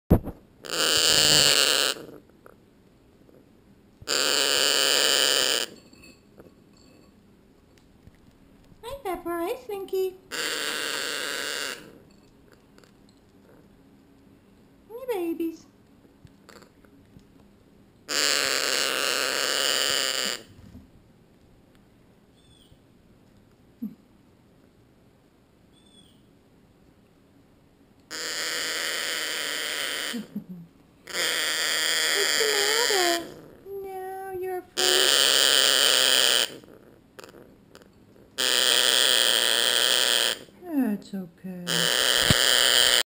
Pepper & Slinky African Grey Timneh family
I had a small video session today with the entire family. Slinky is on the perch.
Pepper is enjoying her nuts and broccoli in the bowl and the little guys are dug deep into the pine bedding for extra warmth. The youngest one did not want to settle down and was growling the entire time while the parents were around.